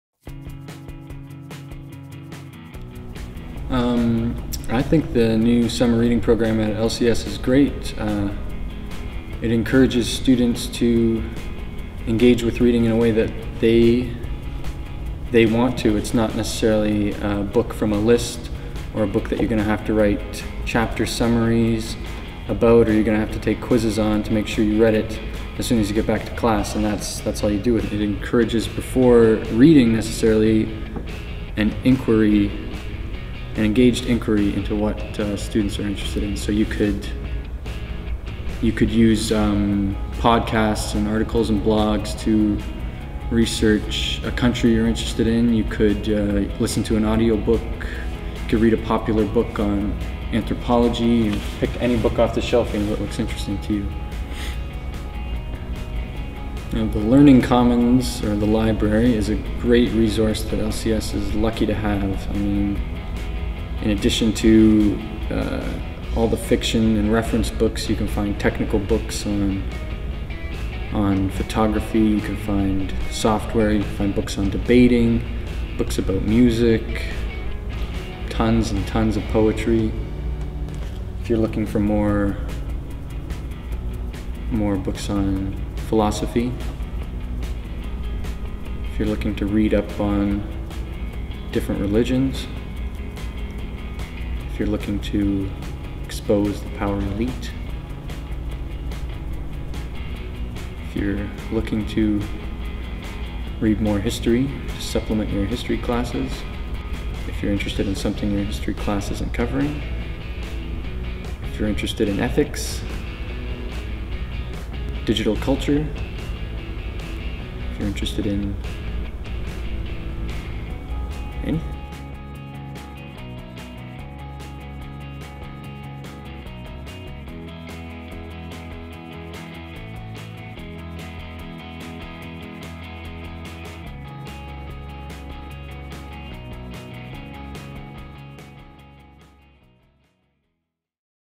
Summer Reading at LCS
Jesse Sarkis ’13 talks about Summer Reading Program and the Phelan Family Learning Commons at LCS. Over the summer LCS students should exercise “voice and choice.” A “one size fits all” book does not exist, for readers have different interests, passions, and experiences. See the links below for more information.

Ms. Brown’s Summer Reading Picks

Orientation Guide (See pages 20-21 for information on the summer reading program and resources at LCS)